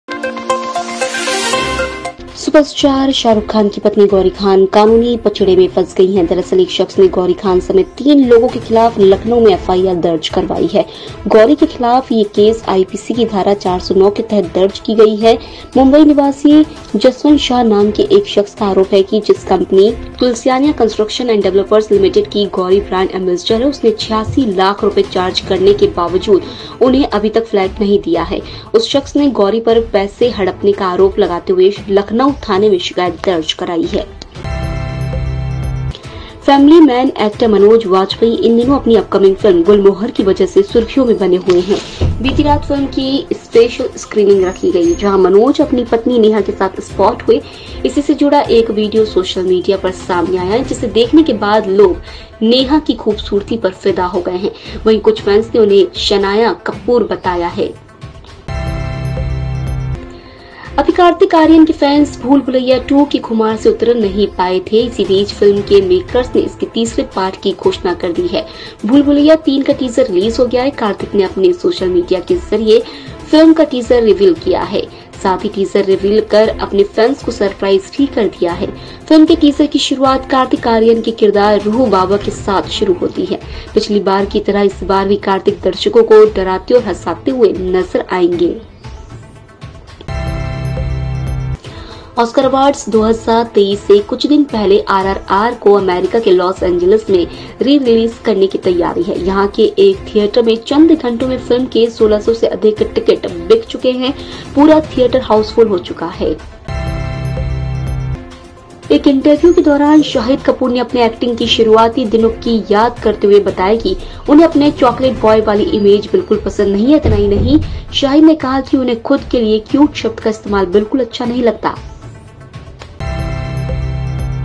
शाहरुख की पत्नी के खिलाफ FIR! गौरी खान पर लगा धोखाधड़ी का आरोप | EMS TV 02-Mar-2023
शाहरुख खान की पत्नी गौरी खान पर लगा धोखाधड़ी का आरोप

सुपरस्टार शाहरुख खान की पत्नी गौरी खान कानूनी पचड़े में फंस गई हैं। दरअसल एक शख्स ने गौरी खान समेत 3 लोगों के खिलाफ लखनऊ में FIR दर्ज करवाई है। गौरी के खिलाफ ये केस आईपीसी की धारा 409 के तहत दर्ज किया गया है। मुंबई निवासी जसवंत शाह नाम के एक शख्स का आरोप है कि जिस कंपनी (तुलस्यानी कंस्ट्रक्शन एंड डेवलपर्स लिमिटेड) की गौरी ब्रांड एंबेसडर हैं, उसने 86 लाख रुपए चार्ज करने के बावजूद उन्हें अभी तक फ्लैट नहीं दिया है। उस शख्स ने गौरी पर पैसे हड़पने का आरोप लगाते हुए लखनऊ थाने में शिकायत दर्ज करवाई है।

मनोज बाजपेयी की पत्नी को देख चौंके फैंस

'फैमिली मैन' एक्टर मनोज बाजपेयी इन दिनों अपनी अपकमिंग फिल्म 'गुलमोहर' की वजह से सुर्खियों में बने हुए हैं। बीती रात फिल्म की फिल्म की स्पेशल स्क्रीनिंग रखी गई, जहां मनोज अपनी पत्नी नेहा के साथ स्पॉट हुए, इसी से जुड़ा एक वीडियो सोशल मीडिया पर सामने आया है, जिसे देखने के बाद लोग नेहा की खूबसूरती पर फिदा हो गए हैं। वहीं, कुछ फैंस ने उन्हें शनाया कपूर बता दिया है।

फिर रूह बाबा बनकर हंसाते-डराते नजर आएंगे कार्तिक आर्यन

अभी कार्तिक आर्यन के फैंस भूल भुलैया 2 के खुमार से उतर नहीं पाए थे। इसी बीच फिल्म के मेकर्स ने इसके तीसरे पार्ट की घोषणा कर दी है। भूल भुलैया 3 का टीजर रिलीज हो गया है। कार्तिक ने अपने सोशल मीडिया के जरिए फिल्म का टीजर रिवील किया है, साथ ही टीजर रिवील कर अपने फैंस को सरप्राइज भी कर दिया है। फिल्म के टीजर की शुरुआत कार्तिक आर्यन के किरदार रूह बाबा के साथ शुरू होती है। पिछली बार की तरह इस बार भी कार्तिक दर्शकों को डराते और हंसाते नजर आएंगे।

ऑस्कर अवॉर्ड्स से पहले अमेरिका में RRR का जलवा

ऑस्कर अवॉर्ड्स 2023 से कुछ दिन पहले RRR को अमेरिका के लॉस एंजिलिस में रि-रिलीज करने की तैयारी है। यहां के एक थिएटर में चंद घंटों में फिल्म के 1600 से अधिक टिकट बिक चुके हैं। पूरा थिएटर हाउसफुल हो चुका है।

शाहिद को नहीं अच्छी लगती अपनी चॉकलेट बॉय वाली इमेज

एक इंटरव्यू के दौरान शाहिद कपूर ने अपने एक्टिंग के शुरुआती दिनों के याद करते हुए बताया कि उन्हें अपनी चॉकलेट बॉय वाली इमेज बिल्कुल पसंद नहीं है। इतना ही शाहिद ने कहा कि उन्हें खुद के लिए क्यूट शब्द का इस्तेमाल बिल्कुल अच्छा नहीं लगता है।